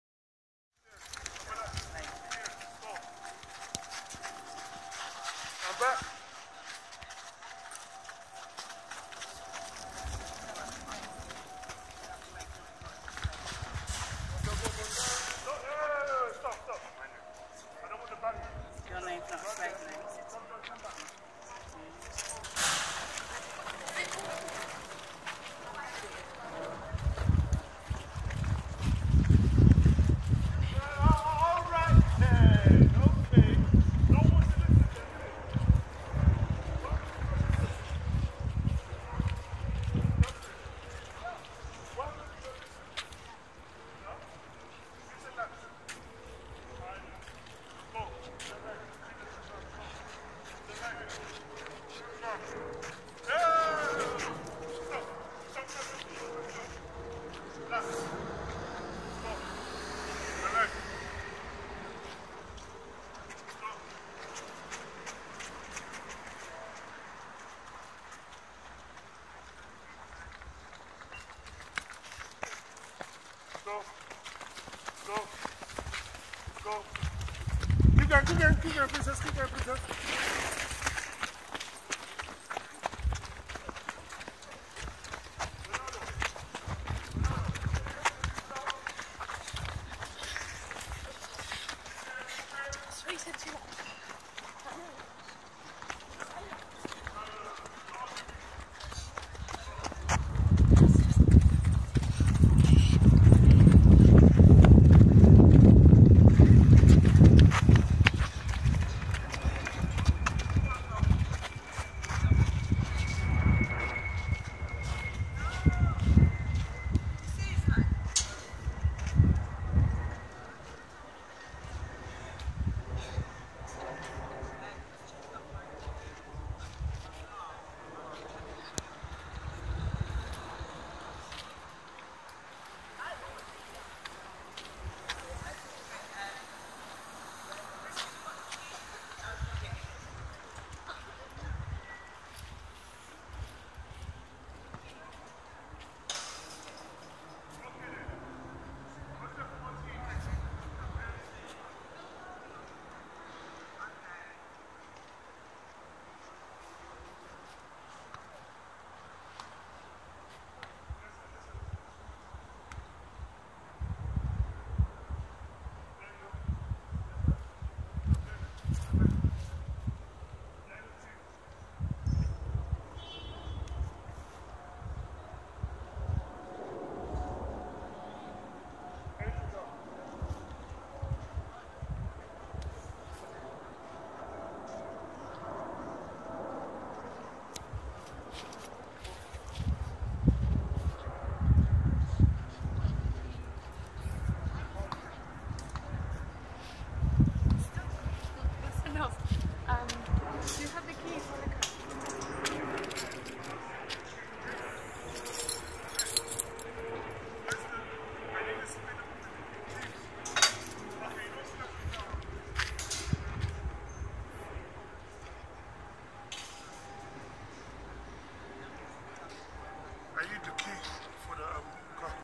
Being Active at CEFLyncx with Grassroots Football
Our Grassroots Football Training Sessions always begin with a good doze harming up exercise; young resident at Angell Town,  Lambeth LOVE It.